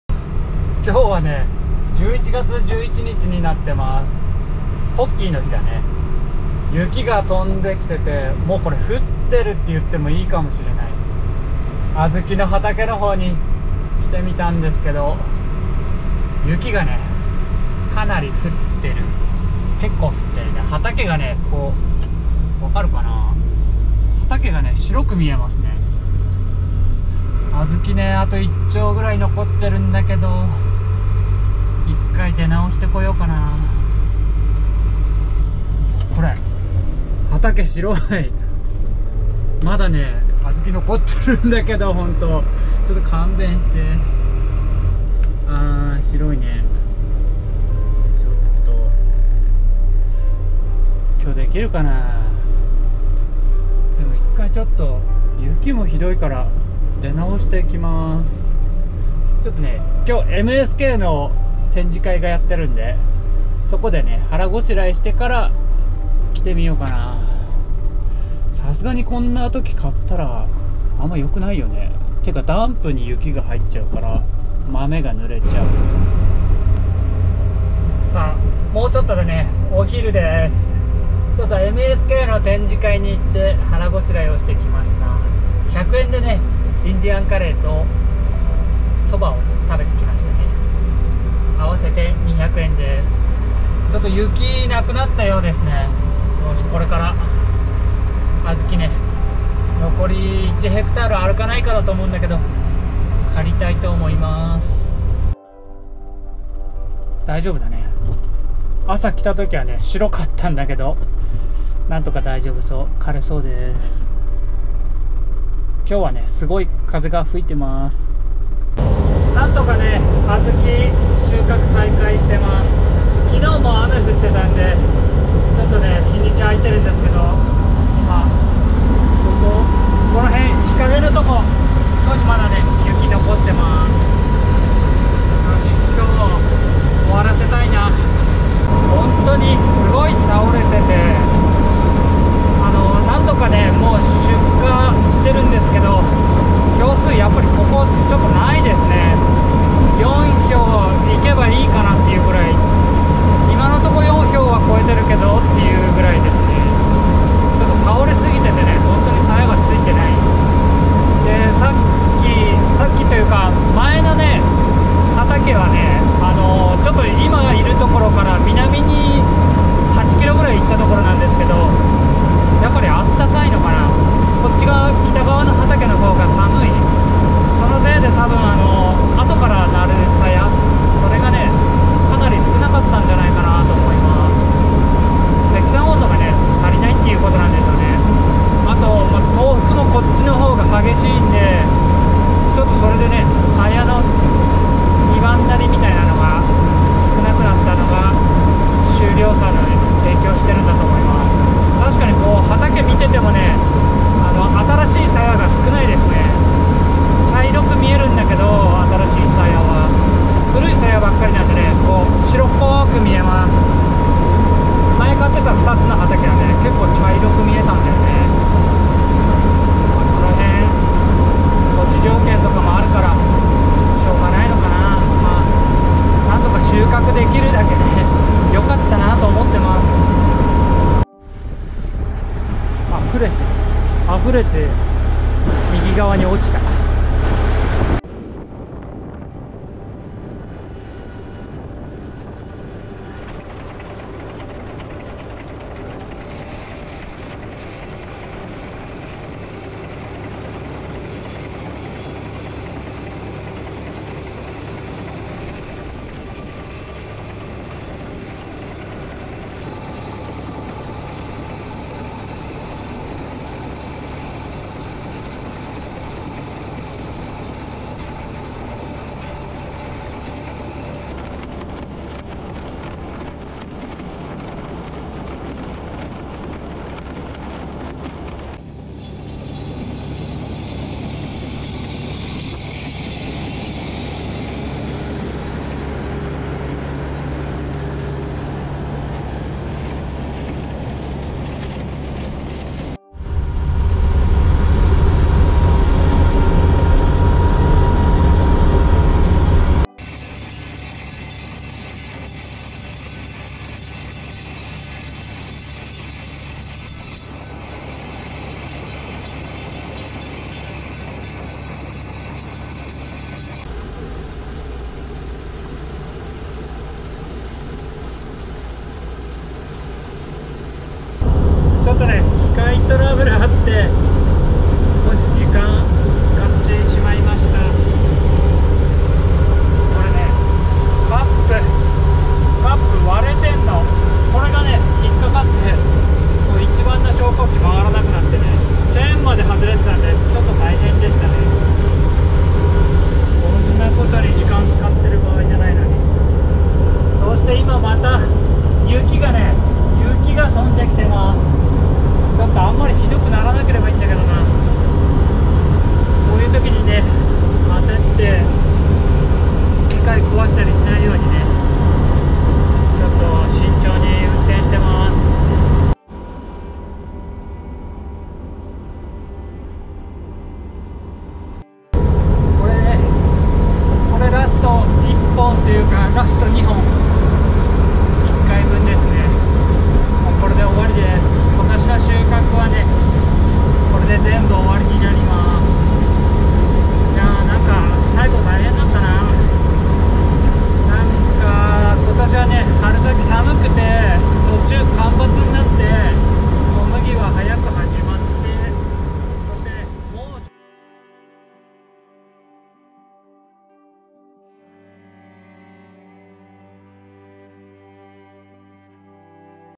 【初雪】オィオィ!!いつまで小豆刈ってんの？
雪が降るほうが早かったかぁ(+o+)

251kg/10a
倒れすぎ···
肥料のやり過ぎ人災だ

ま、なんとか終わって、今も生きているので、なんとかなるべ(･ิω･ิ)??

2024年はもっともーっと減肥しよう

ドウナルコトヤラ(･ิω･ิ)